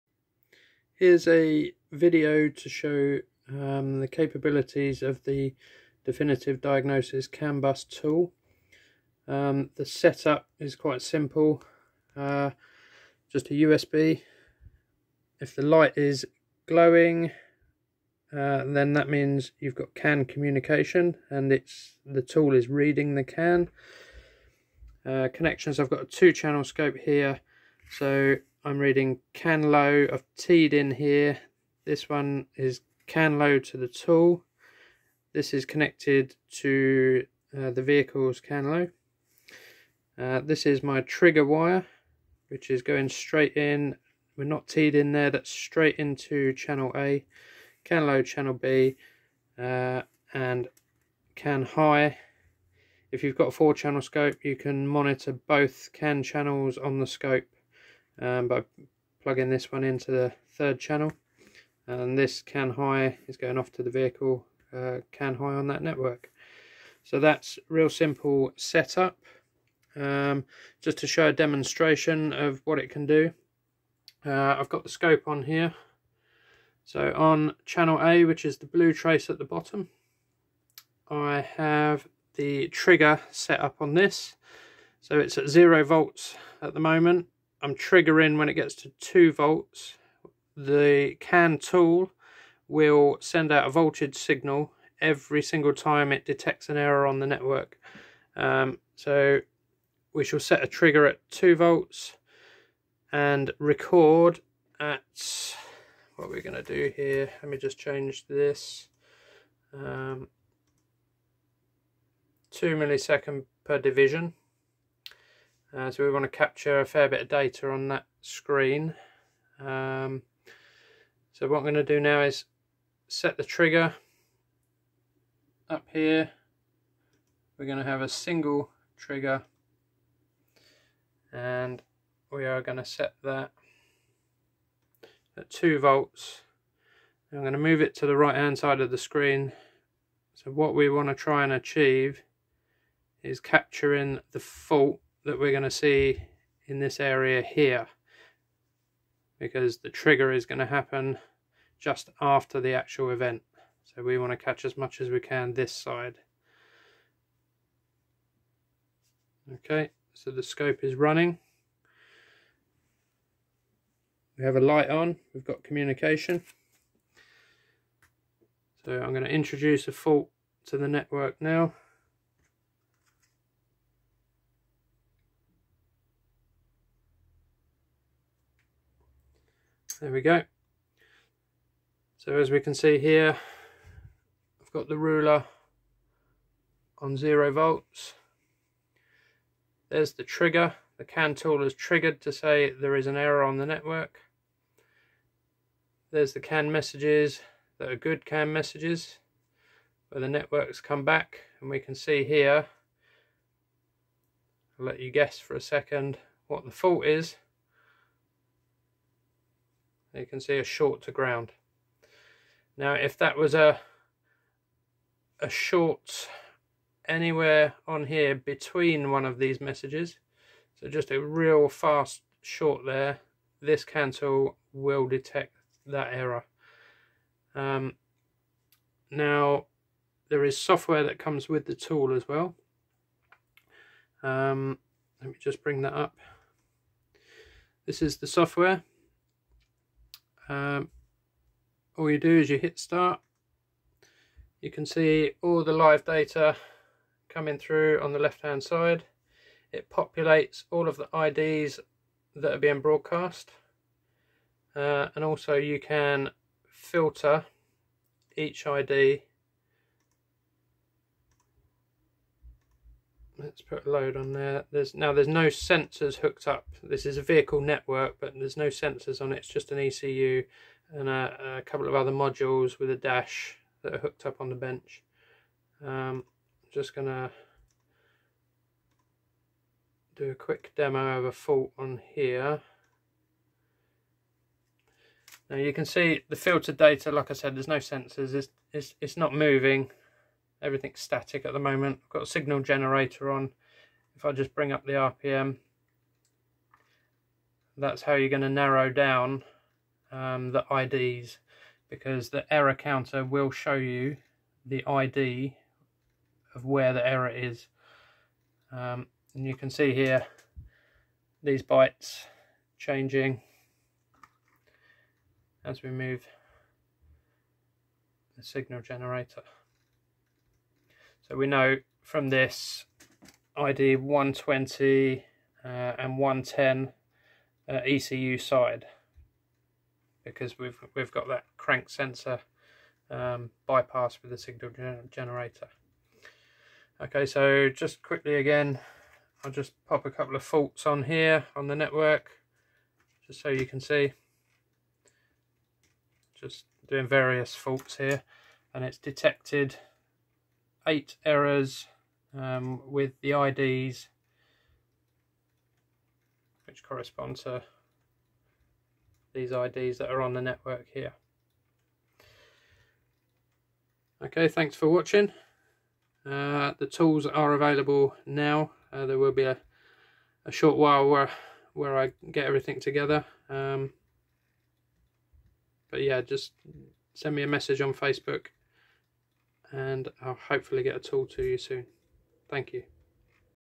CAN Bus tool V2.0 - Demo
This tool decodes CAN 2.0B and now has an output trigger to trigger an oscilloscope to identify intermittent CAN faults.